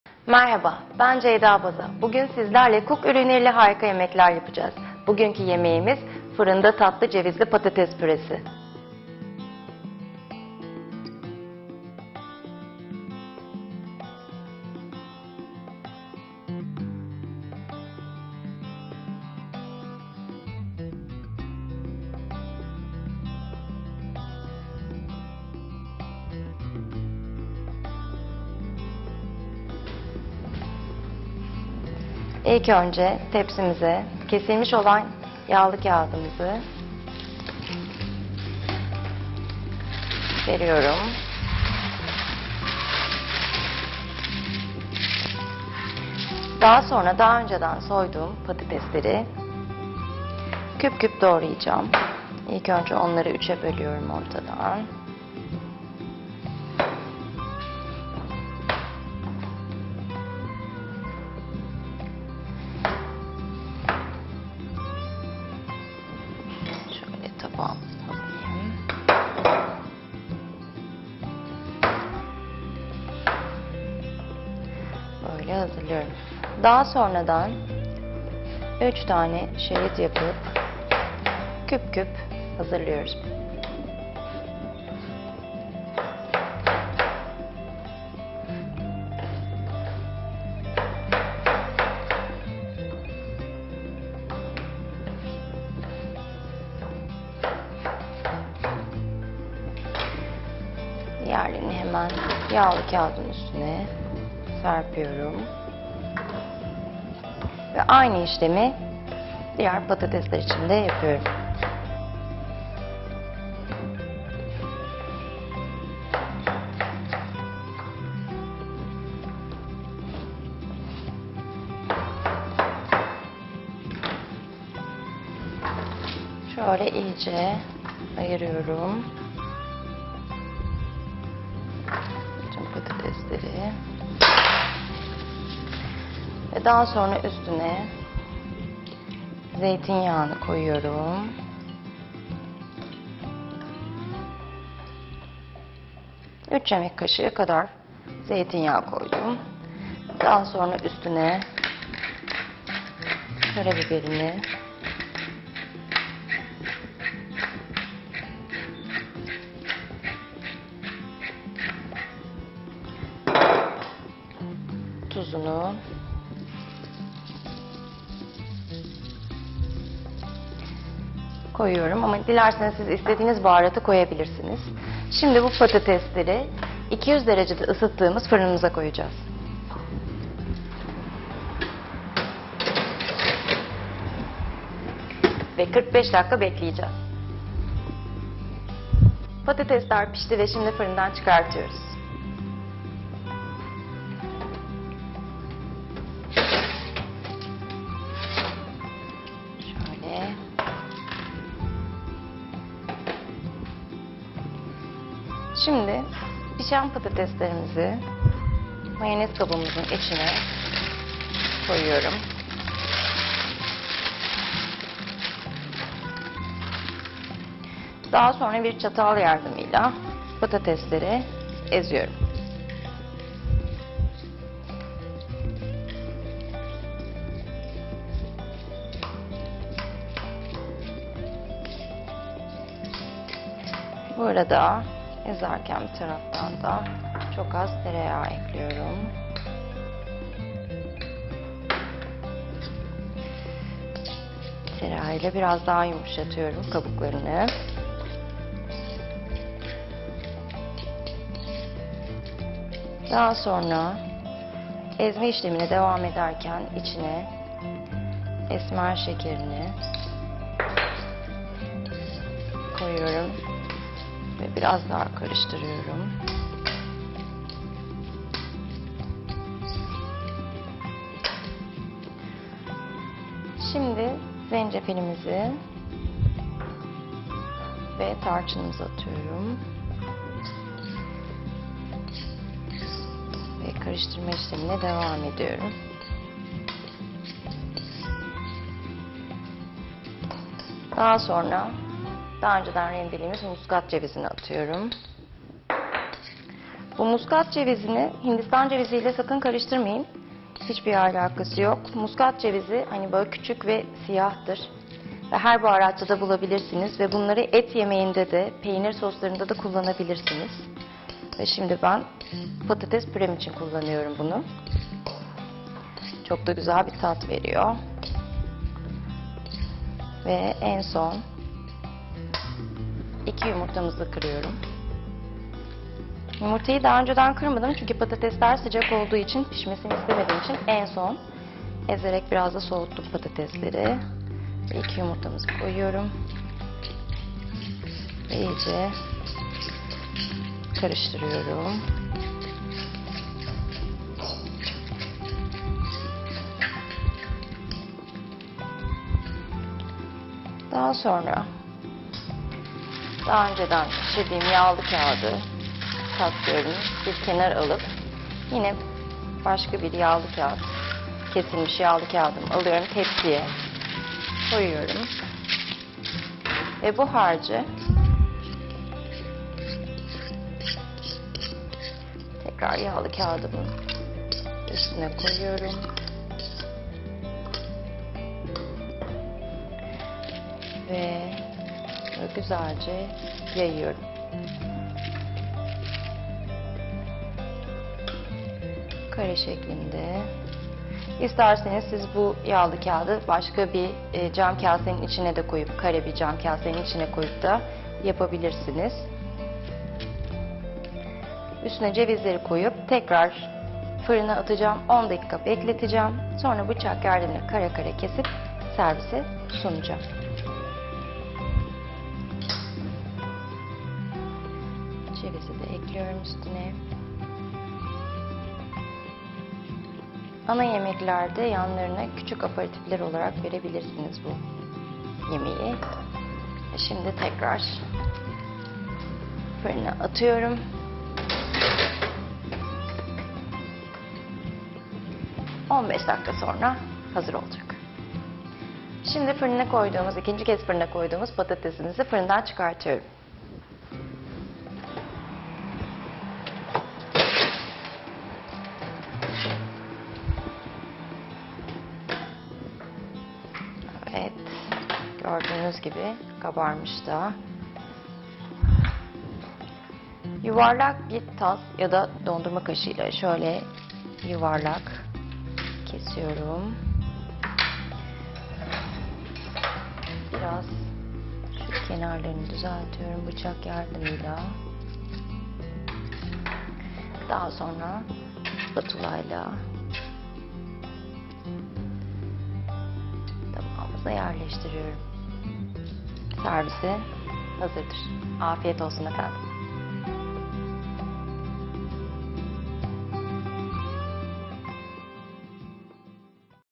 Fırında Tatlı Cevizli Patates Püresi Tarifi
Şef Ceyda Baza COOK Mutfağı'nda Fırında Tatlı Cevizli Patates Tarifinin nasıl yapıldığını anlatıyor.
Diğer videolar ve tarifler için adresimizi ziyaret edin.